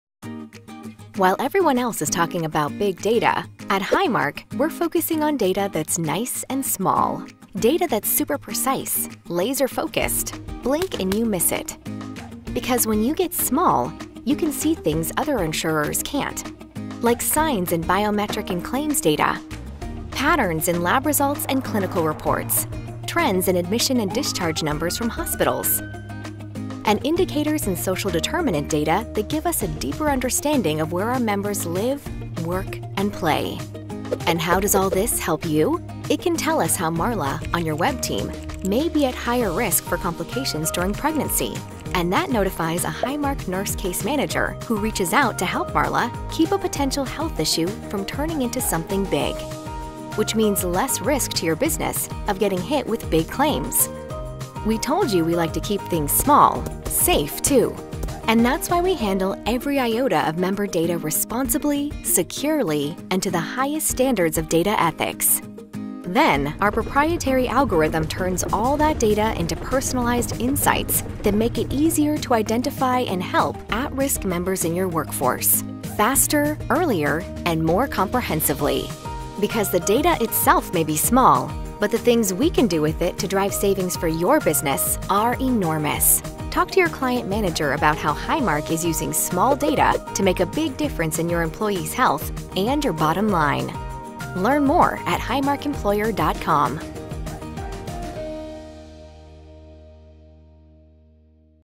Clinical Innovation | High Cost Claims | Client | Highmark
How we’re using data to improve care and lower the number of high cost claimants.